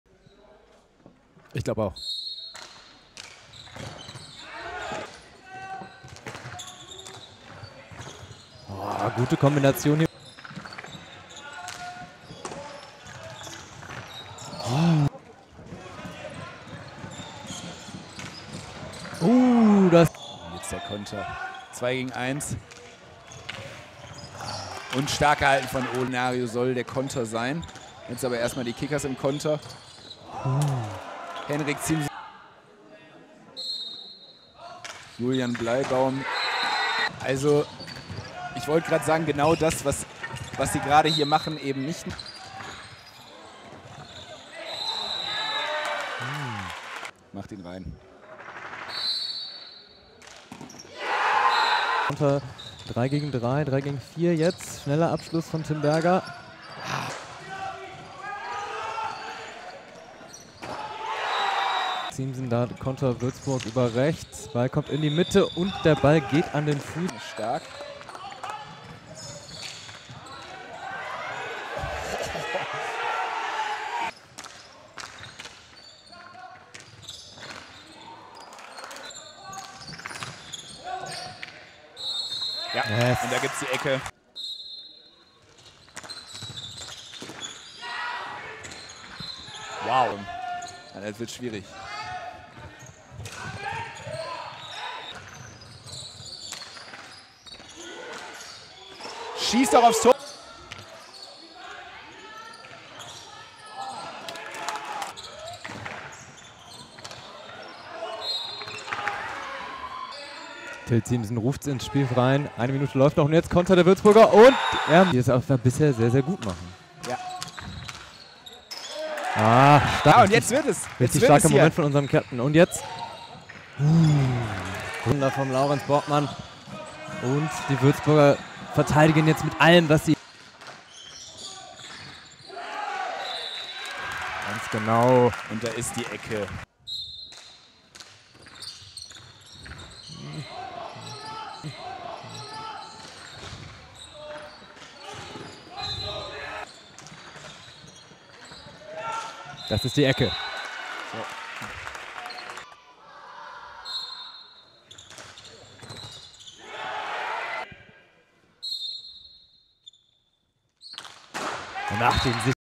Highlights HTC Stuttgarter Kickers - HTC Würzburg
Highlights des Spiels HTC Stuttgarter Kickers - HTC Würzburg am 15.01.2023
2. Bundesliga Süd Saison 2022/2023